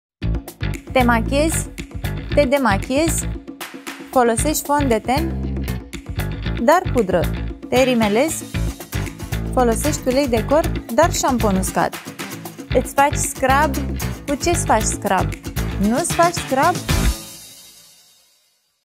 Cosmia beauty tips
Cum alegi uleiul de corp potrivit? Dar șamponul uscat? Care sunt pașii pentru o demachiere corectă? Află de la Oana, specialist în beauty & skin care, răspunsurile la cele mai importante întrebări despre îngrijirea corporală și criteriile de care să ții cont atunci când îți alegi produsele potrivite. Obiceiurile bune încep zilnic, cu Cosmia Romania.